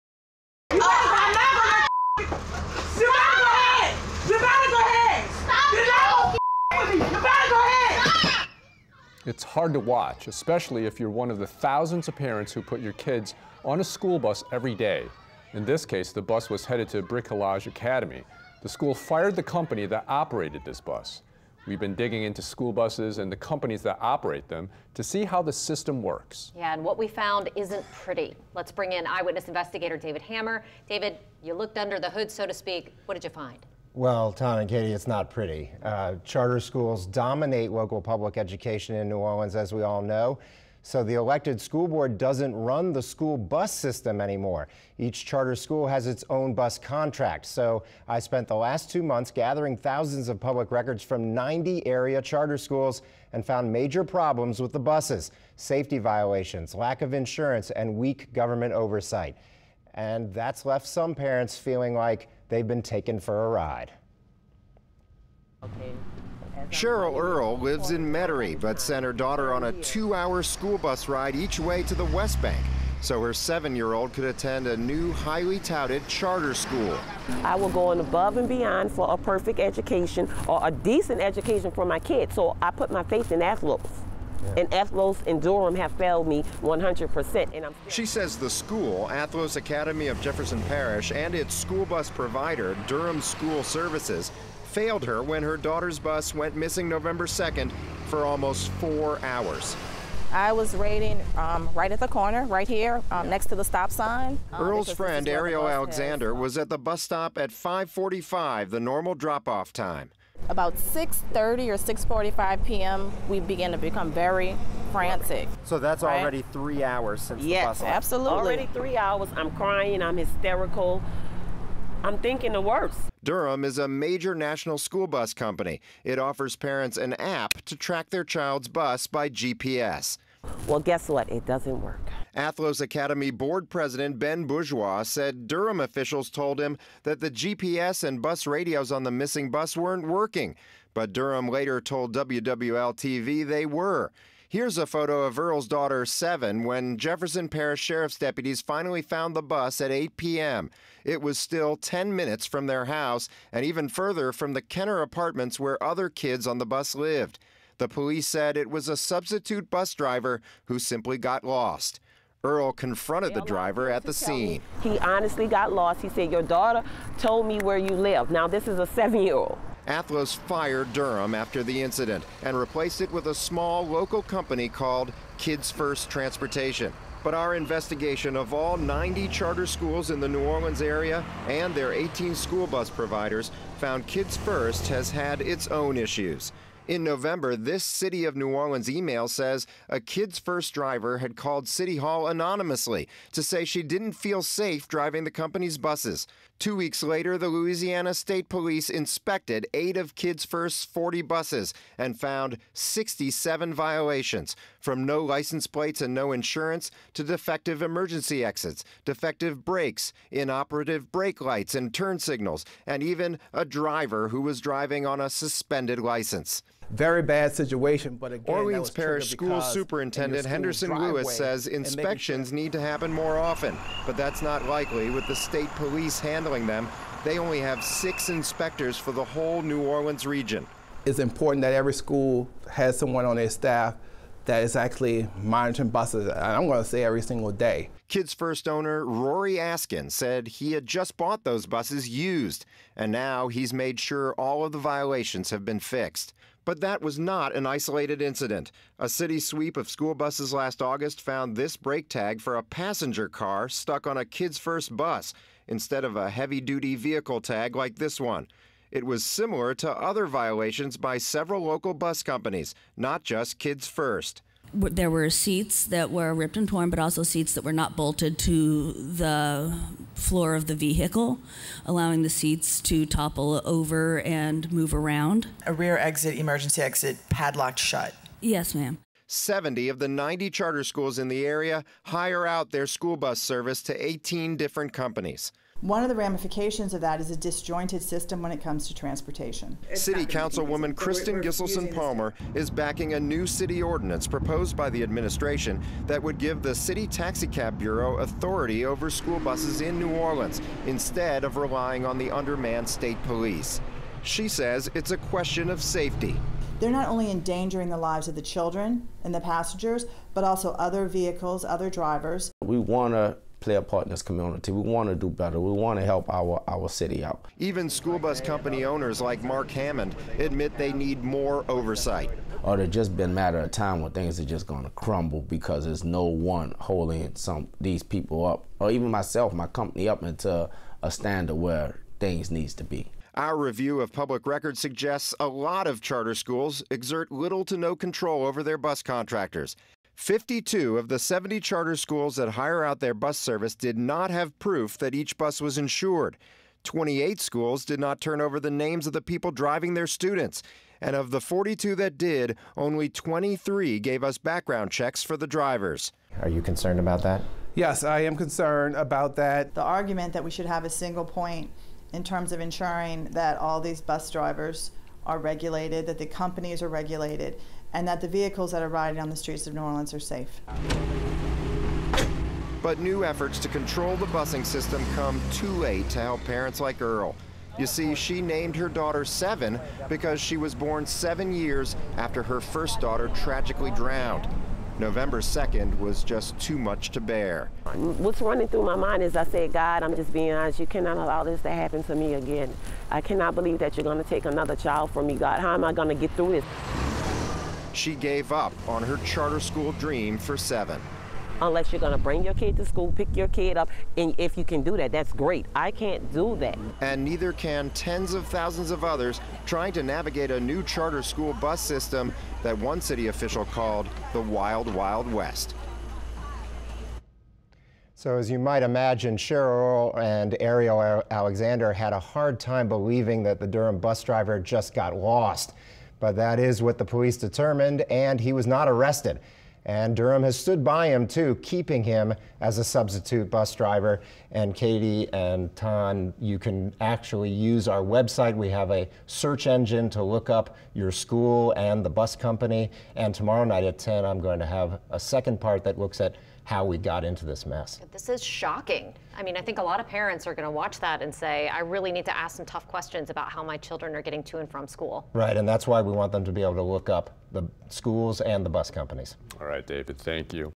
Taken for a Ride: How safe are charter school buses?
Fifty-four of the 70 charter schools that hire out their bus service did not provide WWL-TV proof that each bus was properly insured.